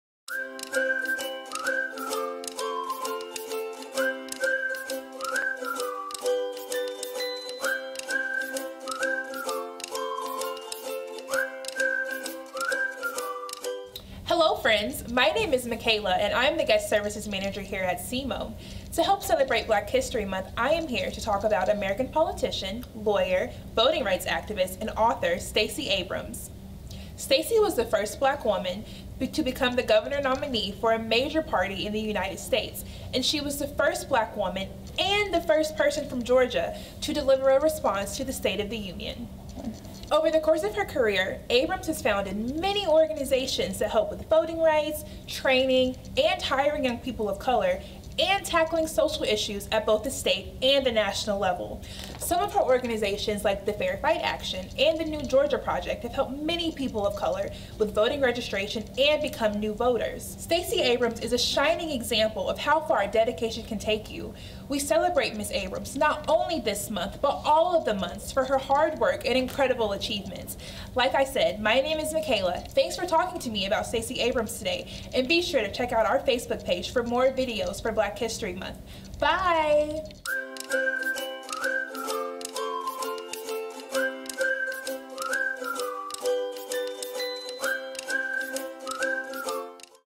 Black History Month (Stacey Abrams)
February is Black History Month and we're going to feature a new change-maker during each cMoe at Home video!

Today we are highlighting the work of Stacey Abrams. Stacey Abrams is an American politician, lawyer, voting rights activist, and author who served in the Georgia House of Representatives.